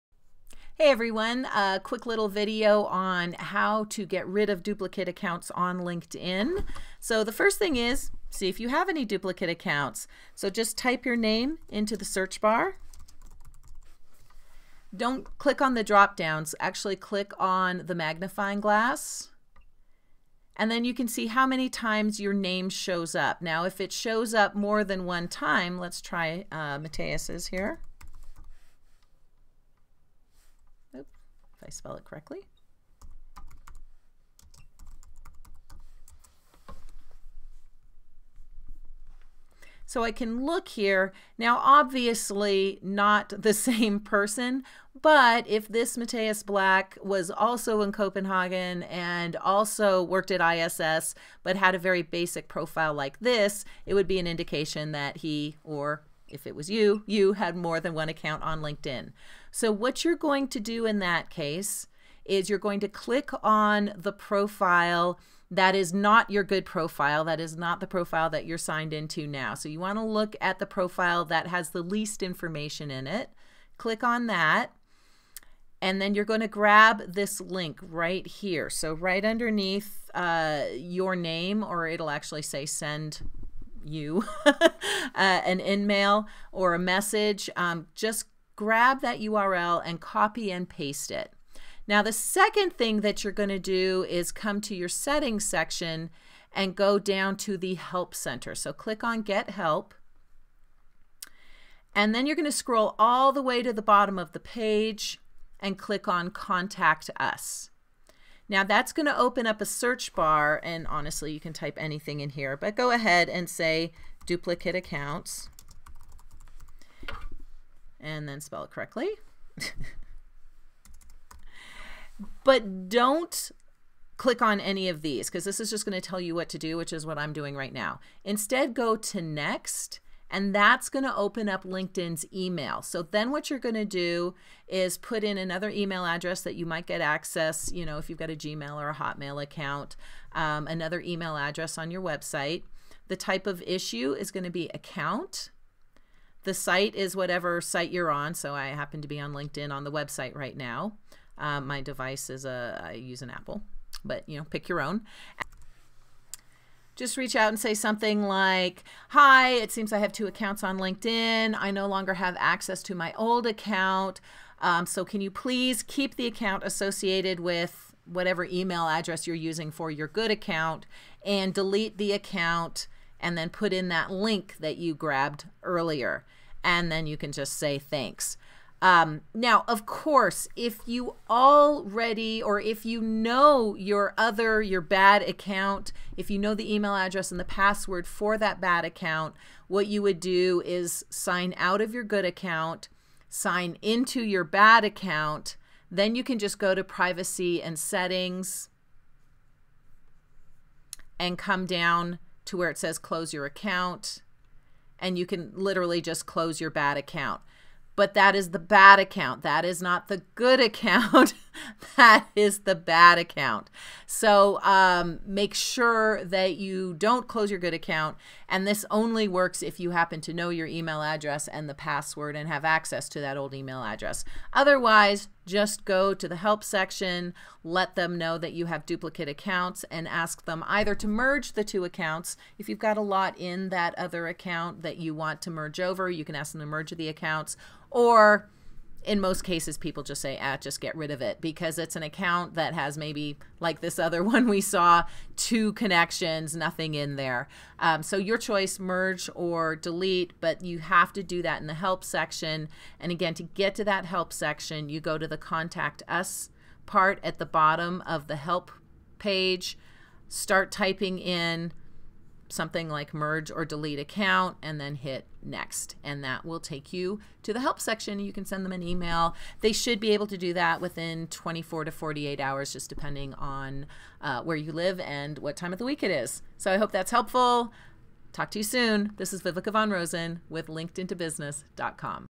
How to Close or Merge Your Account on LinkedIn using the New User Interface
LinkedIn's newest user interface hides the email function - but it is still there if you know where to look!!!